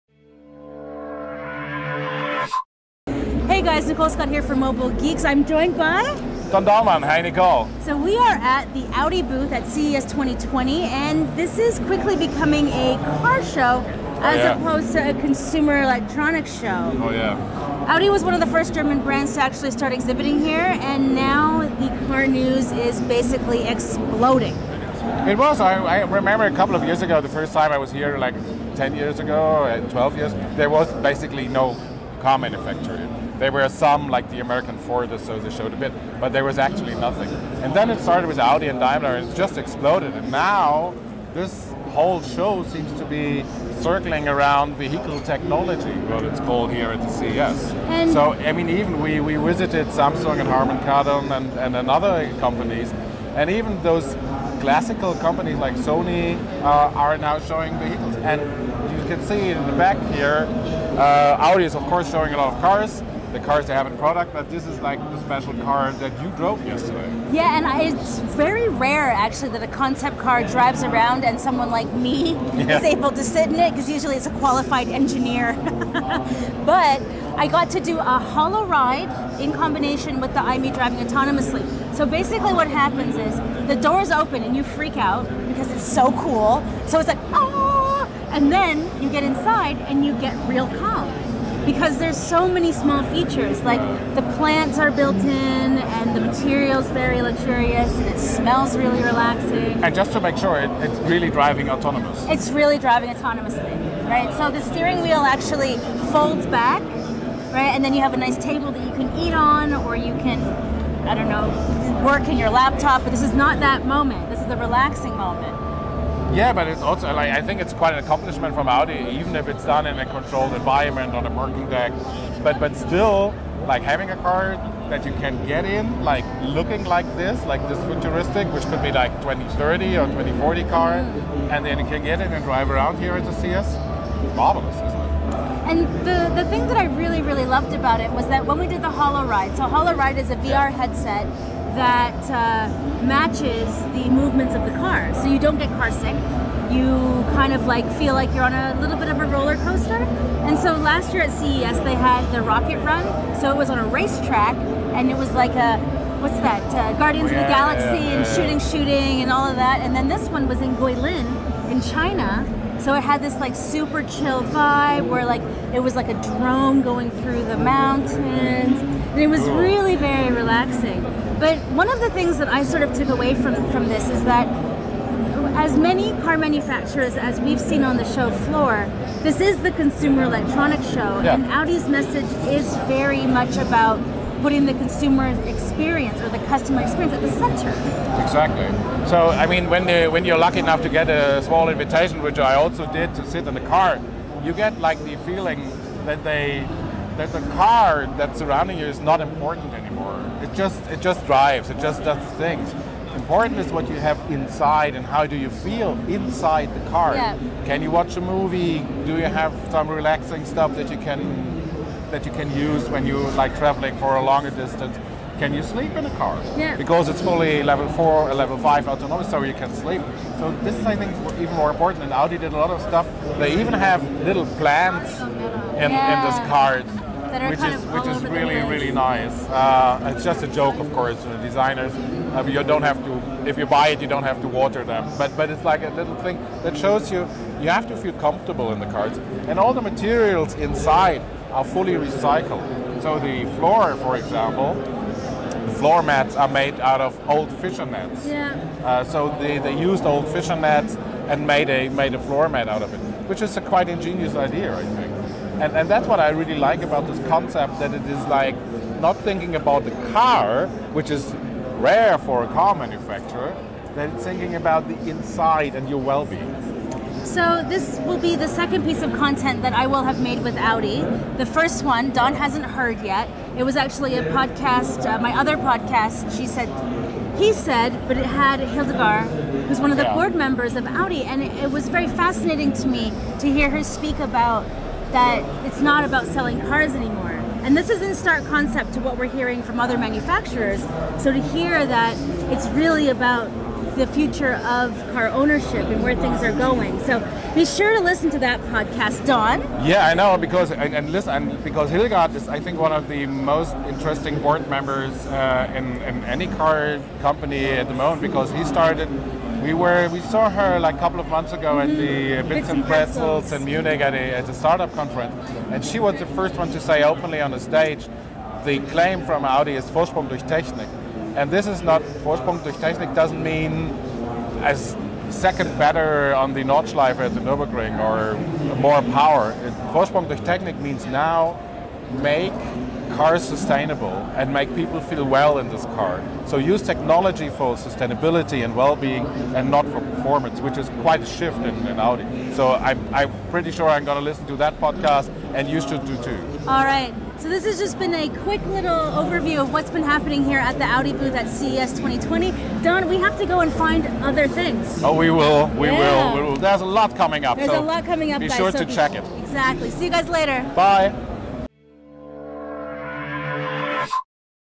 Has CES finally become a car show? A look at the Audi AI:ME
CES has seen more and more car releases, and we're seeing a shift towards vehicle technology. Sony and Amazon both showed off concept cars, Mercedes had a keynote and Audi showed off the AI:ME which drove autonomously while doing a HoloRide demo.